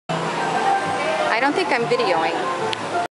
Carolsel ride at the zoo
Kids riding the carosel at the zoo, when Omi and Opa visited in Aug 2012